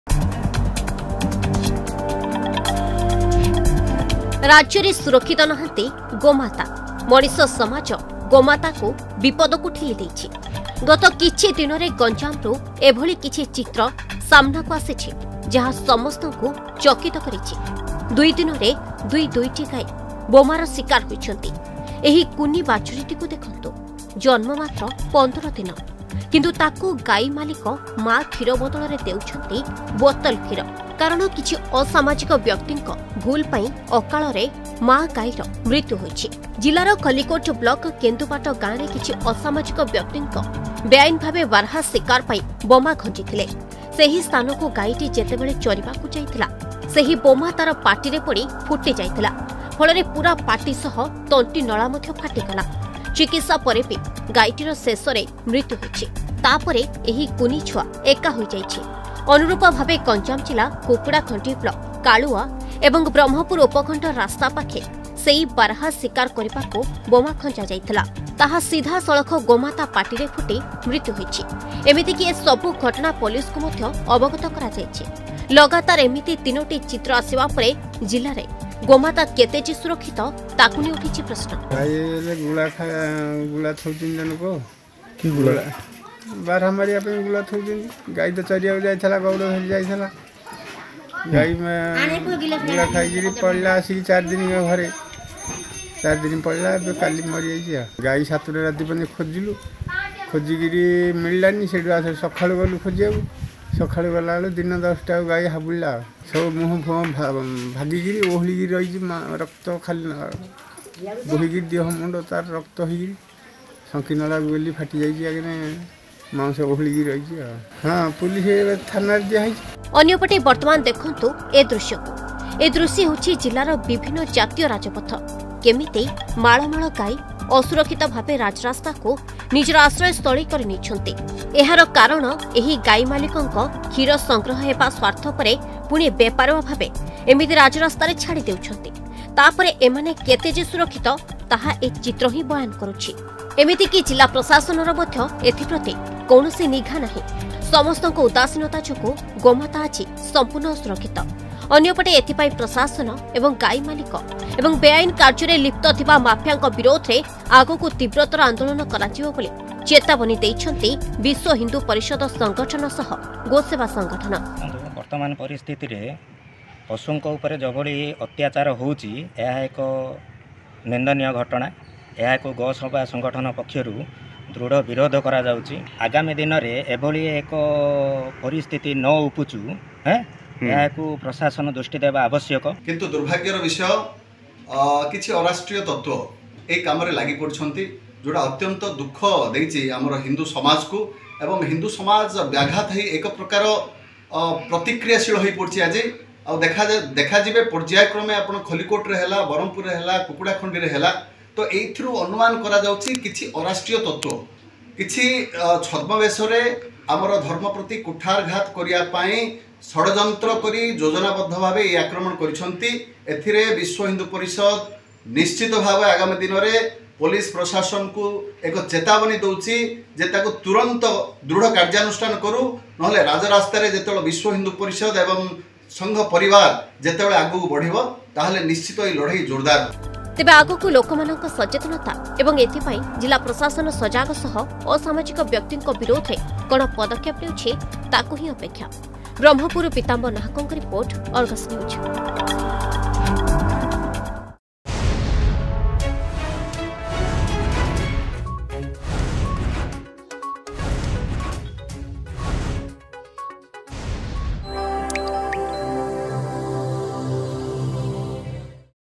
ରାଜ୍ୟରେ ସୁରକ୍ଷିତ ନାହାନ୍ତି ଗୋମାତା
ଗଞ୍ଜାମରେ ଅସୁରକ୍ଷିତ ହୋଇପଡିଲେଣି ଗୋ ମାତା ବେଆଇନ ଭାବେ ବାରହା ଶିକାର ପାଇଁ ବୋମା ଖଂଜୁଥିବା ବ୍ୟକ୍ତିଙ୍କ ଯୋଗୁଁ ଜମିରେ ପଡିଥିବା ବୋମା ଖାଇ ଗୋଟିଏ ପରେ ଗୋଟିଏ ଗୋ ମାତା ଗୁରୁତର ହୋଇପଡୁଛନ୍ତି। ଗଞ୍ଜାମ ଜିଲ୍ଲାରୁ ଦୁଇ ଦିନରେ ଦୁଇ ଦୁଇଟି ଚିତ୍ର ସାମ୍ନାକୁ ଆସିବା ପରେ ଗୋ ମାତାର ସୁରକ୍ଷାକୁ ନେଇ ପ୍ରଶ୍ନବାଚୀ ସୃଷ୍ଟି ହୋଇଛି.